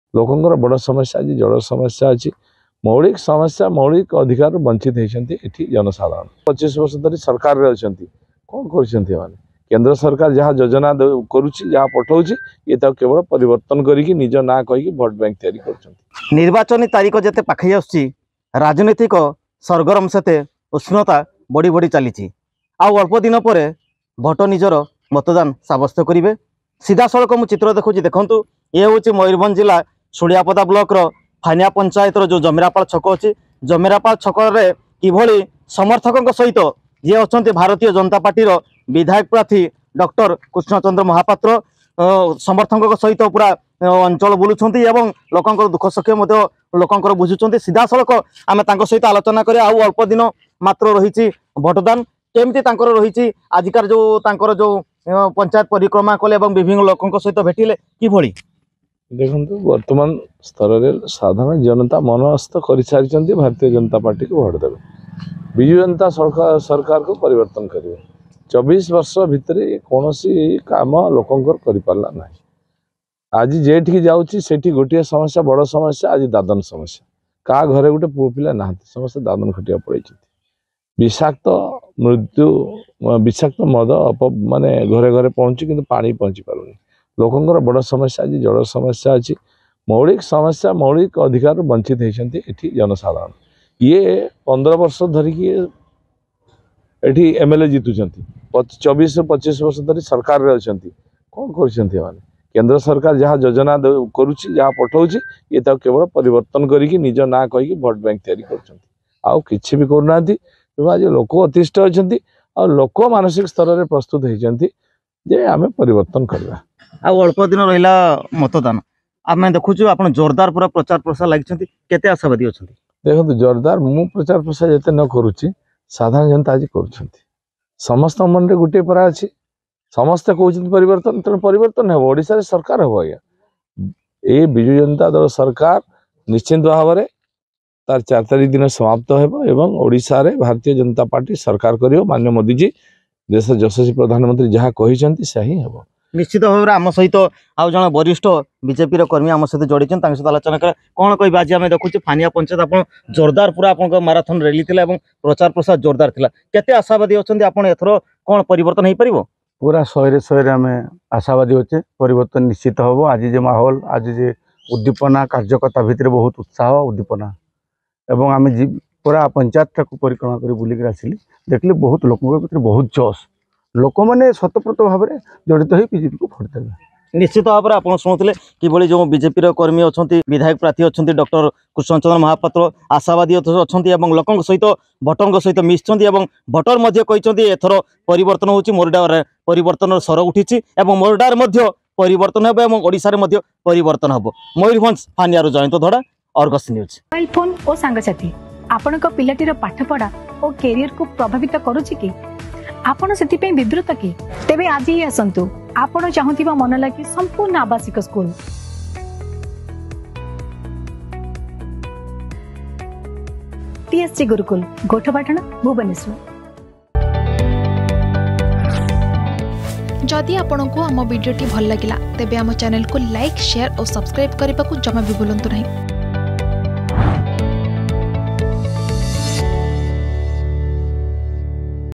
ମୋରଡ଼ା ବିଧାୟକ ପ୍ରାର୍ଥୀ କୃଷ୍ଣଚନ୍ଦ୍ର ମହାପାତ୍ରଙ୍କ ନିର୍ବାଚନୀ ପ୍ରଚାର
ମୋରଡ଼ା ବିଜେପି ବିଧାୟକ ପ୍ରାର୍ଥୀ କୃଷ୍ଣଚନ୍ଦ୍ର ମହାପାତ୍ର ଶୁଳିଆପଦା ବ୍ଲକ କୋଷ୍ଠା ଓ ଫାନିଆ ପଞ୍ଚାୟତରେ ଗାଁ ଗାଁ ବୁଲି ଲୋକଙ୍କ ସହିତ ଭେଟି ନିଜର ନିର୍ବାଚନୀ ପ୍ରଚାର କରୁଛନ୍ତି ।